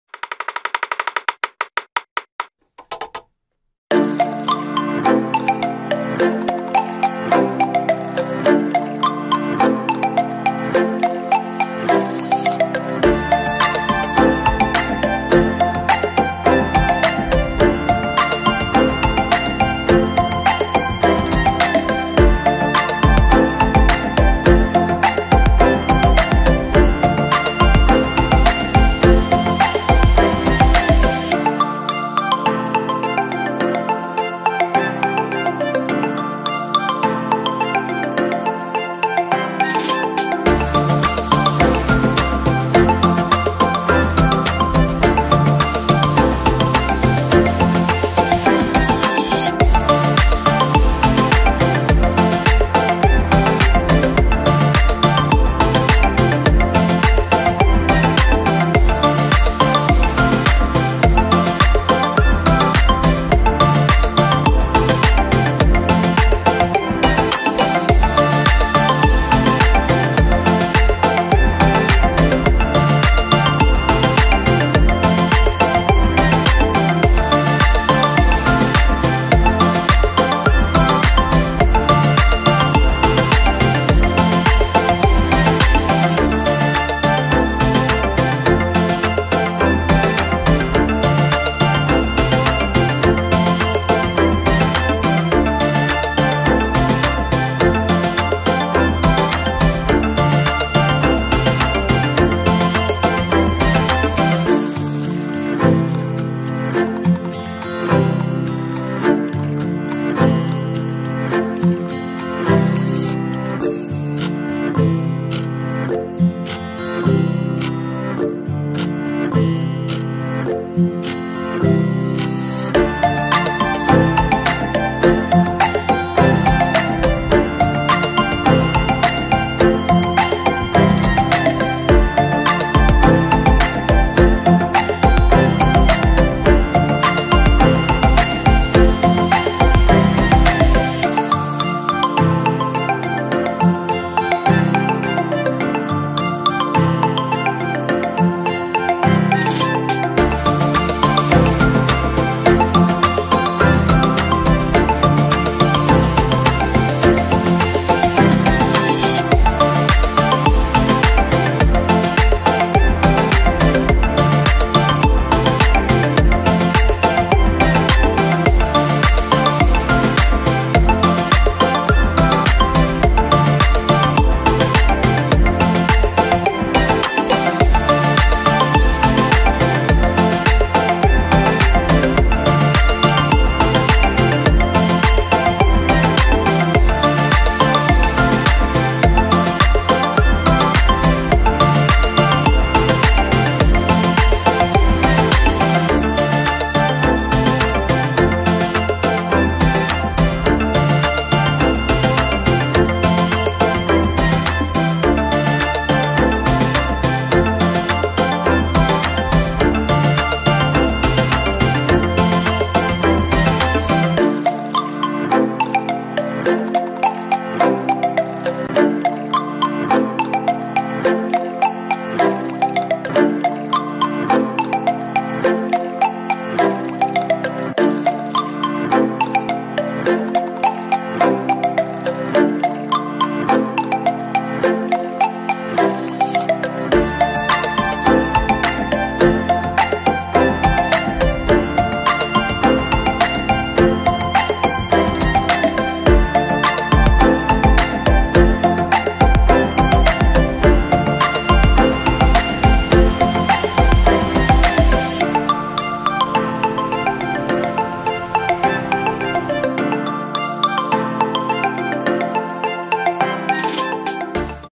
Gaya Rambut LEGENDA RAPI | Potong Rambut Model Mandarin
video kali ini seorang customer ingin potong rambut model mandarin. model mandarin merupakan gaya rambut yang sangat melegenda kerapiannya. mari simak videonya. terima kasih.